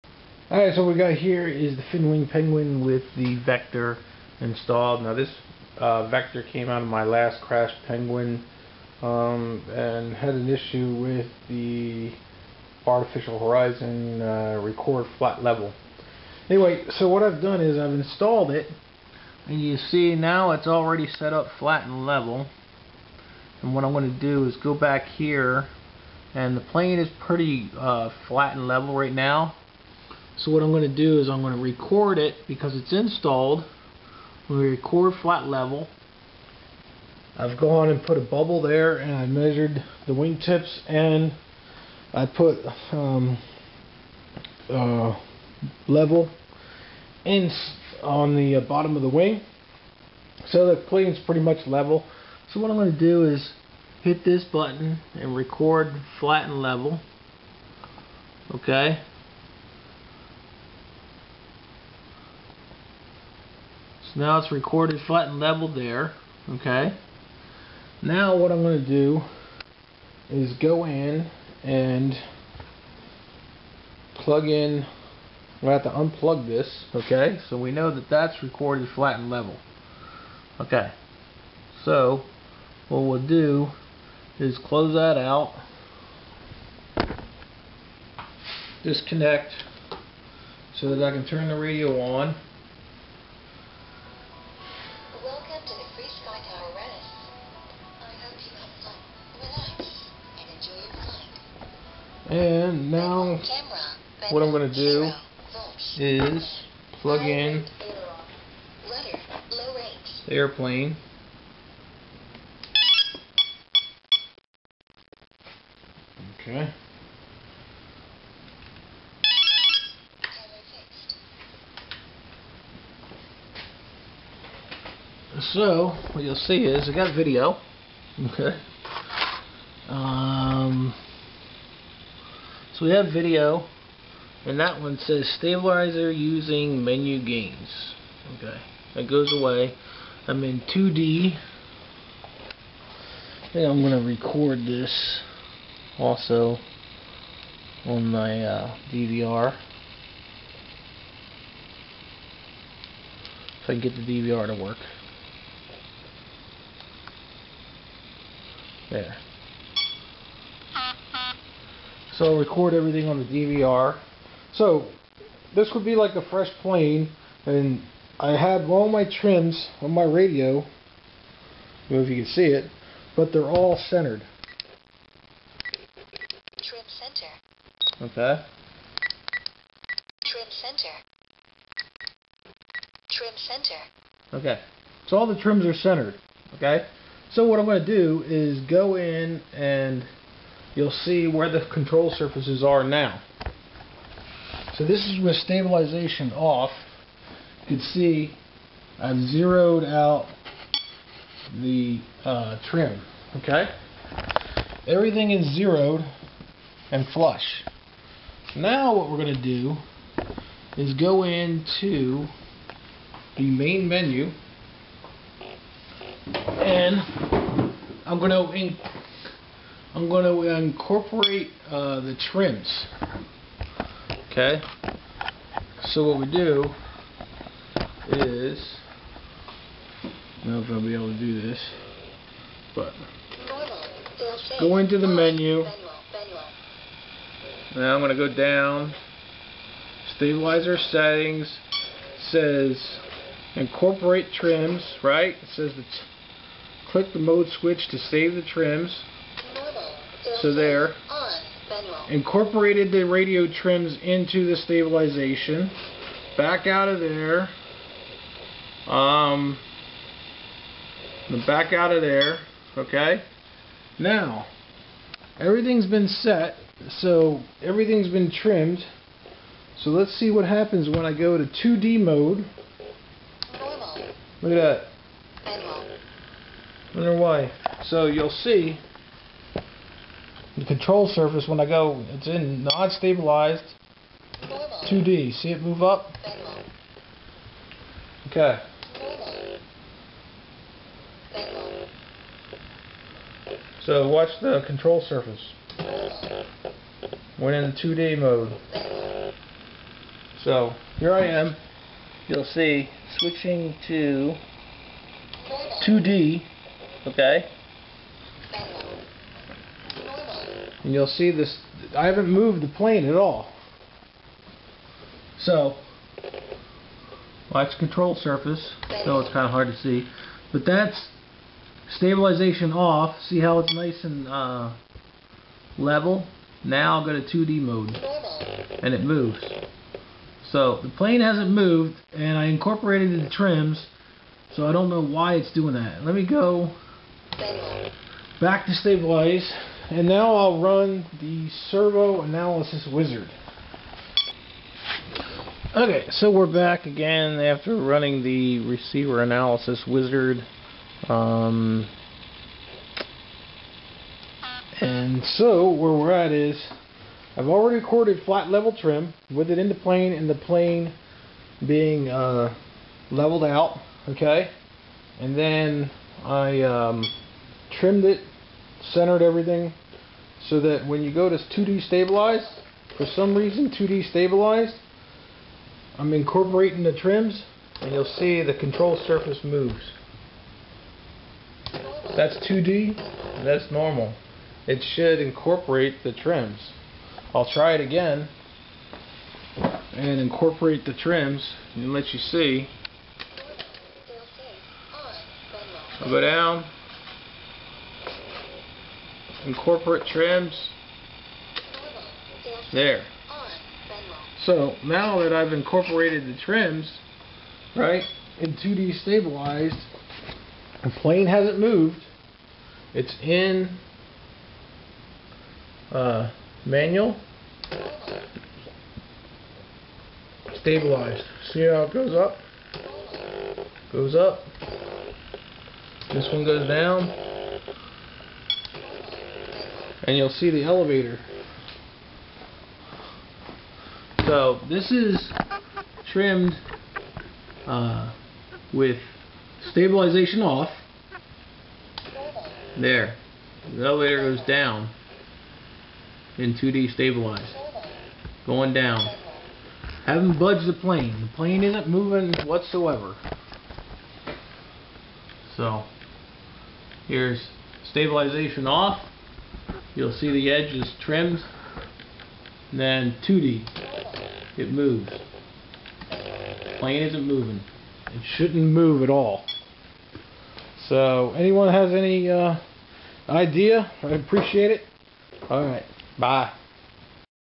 Eagle Tree Vector Issues with Incorporating Trims in 2D mode
DeI just finished installing the Vector in another Penguin; leveled the plane and recorded flat level and re-zeroed gyros in the software. The radio trims were centered in Stab off; went into the menu and incorporated the trims. The aileron control surface is set flush. When switching from Stab off to 2D mode, the left aileron moves up about 1/8"; the right aileron moves down and the elevator moves down. The plane is sitting on the bench not being moved whatsoever. I then performed the Rx wizard and incorporated the trims again via the menu and nothing changed; surfaces still move when in 2D mode when the plane is remaining still.  Any ideas?